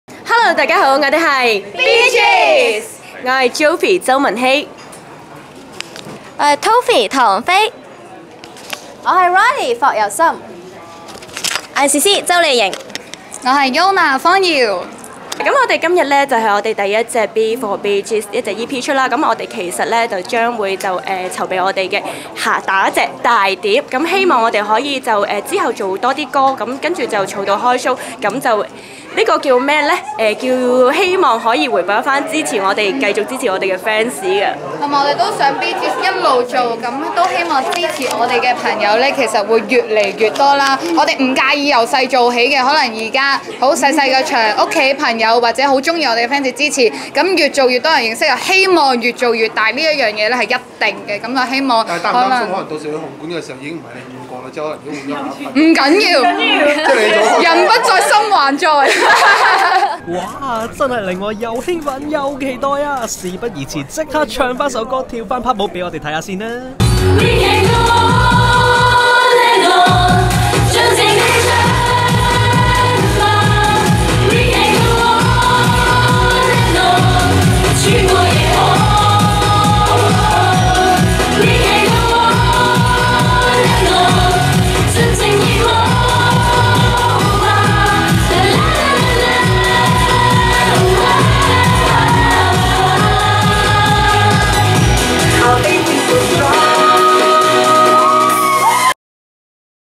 簽唱會疑咪嘴！B.Gs晒腰跳扭Pat趷籮勁舞
B.Gs
鄒莉盈（Ceci）、方堯（Yona）、周汶希（Jovi）、霍柔心（Riley）及唐菲（Toffee）